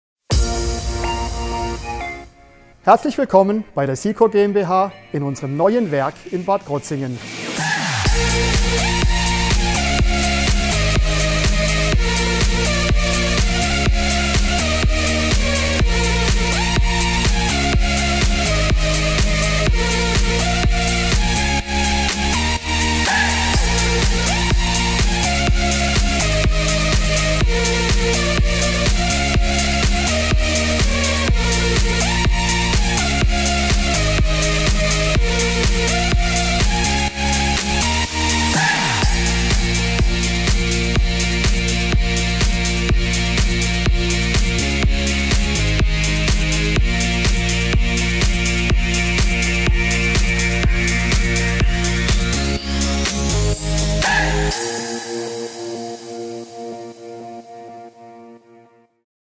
SIKO Bad Krozingen - Der neue coole Standort
Auf 5000 m² Nutzfläche sind nun im neuen Standort Bad Krozingen die Produktion der elektronischen Geräte sowie Vertrieb und Marketing tätig. Das Investitionsvolumen inklusive Maschinen und Gebäudeausstattung betrug 13 Millionen Euro und ist ein Bekenntnis zur lokalen Produktion in Deutschland und zu den treuen und langjährigen Mitarbeiterinnen und Mitarbeitern aus der Region.